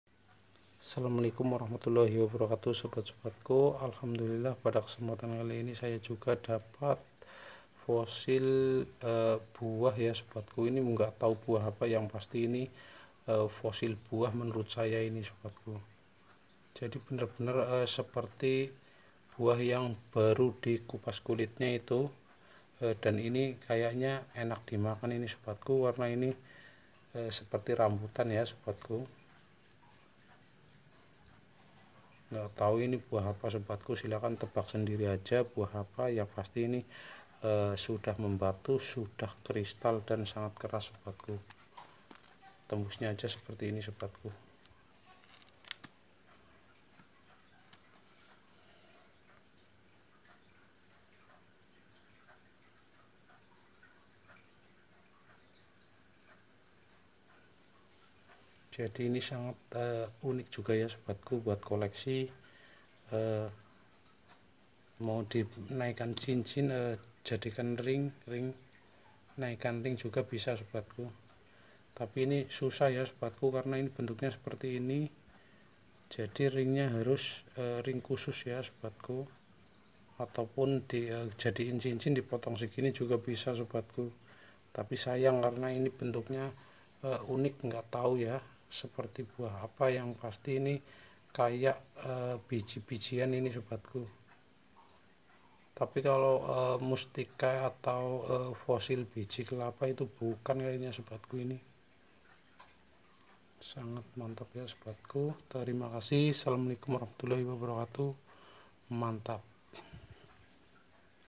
INI BARU FOSIL BUAH
ini baru fosil buah ya sobatku mantap sekali barang sangat langka sekali karena barang ini di buru para kolektor